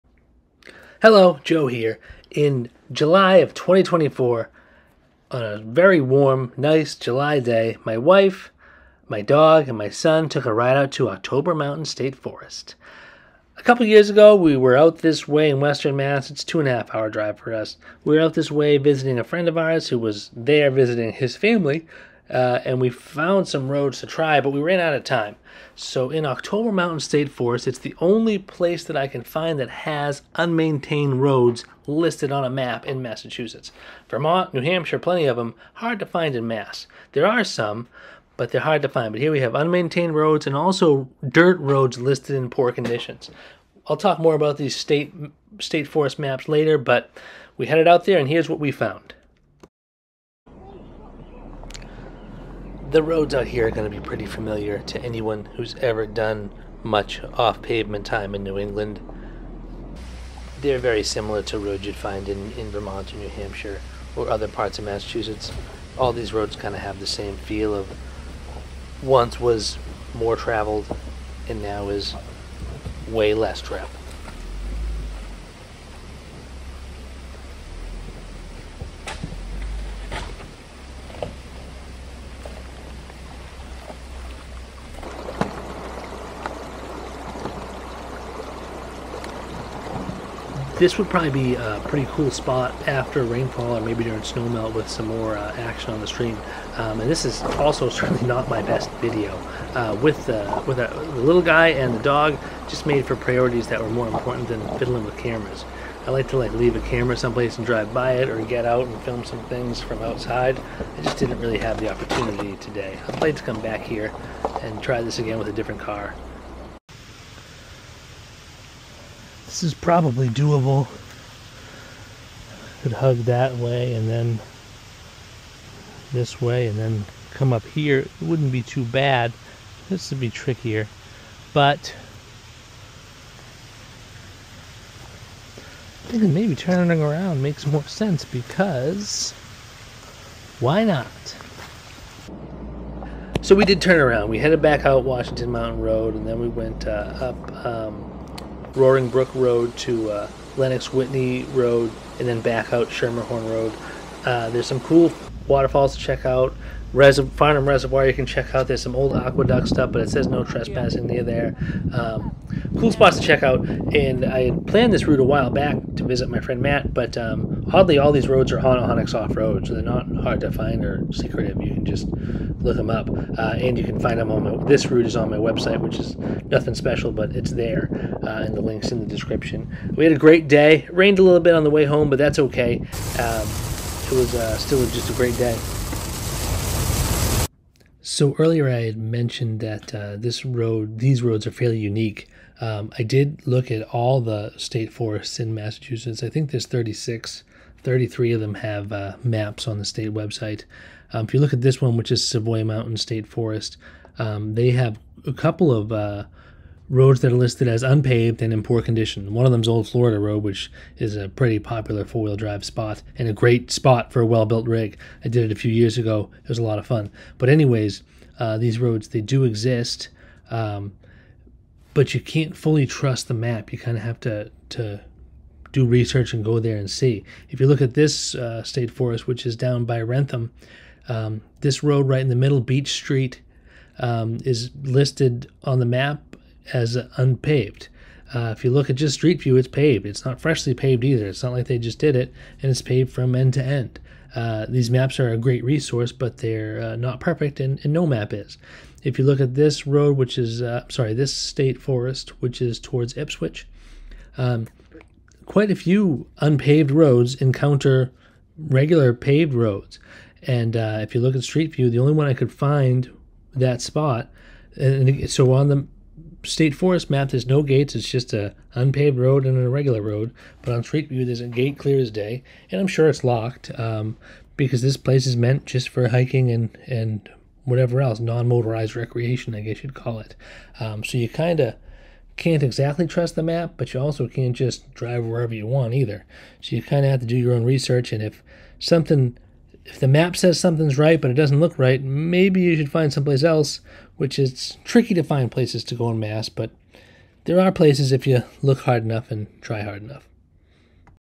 Quick Look at October Mountain State Forest
In July of 2024 went out to explore some unmaintained roads in October Mountain State Forest out in Western Mass.